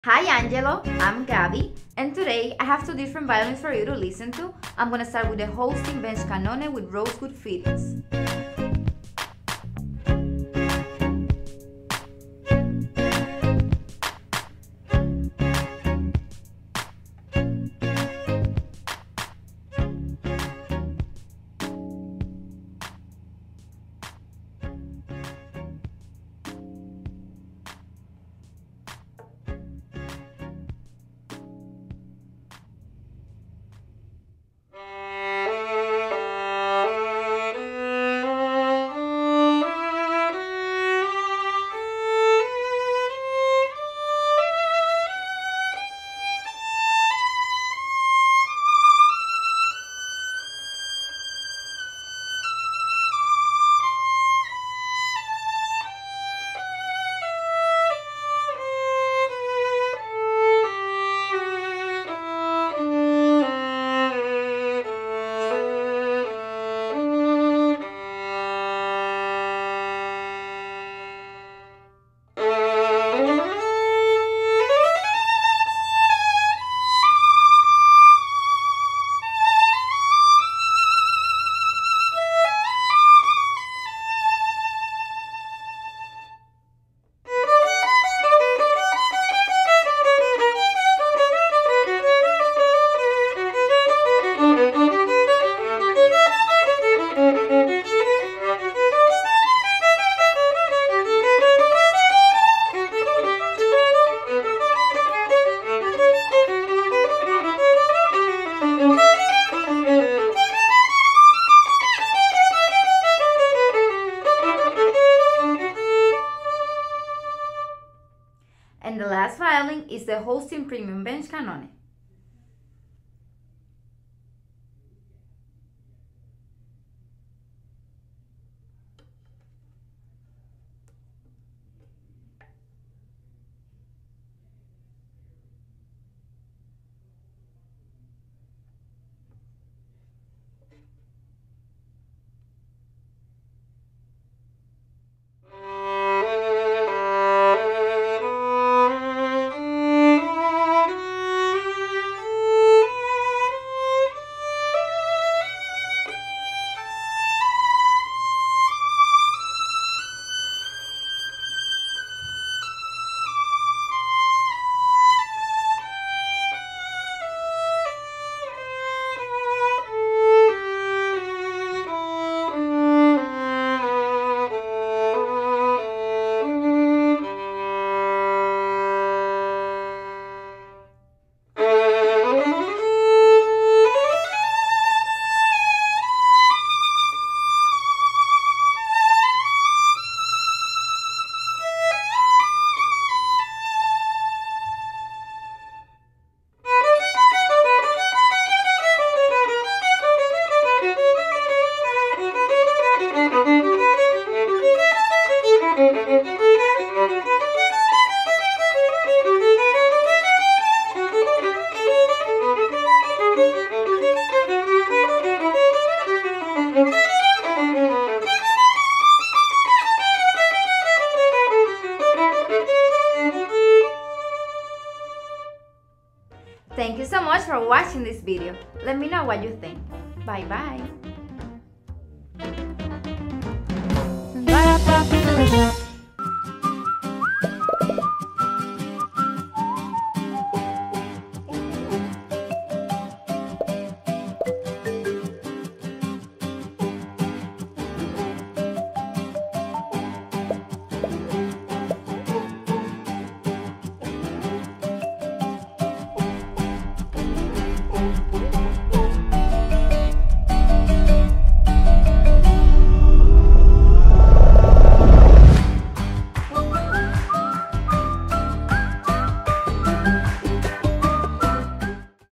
Holstein Bench Cannone w/ Rosewood Fittings and Holstein Premium Bench Cannone Violins for Angelo
This video is for our dear customer Angelo, but for everyone else, you can learn more about these instruments here: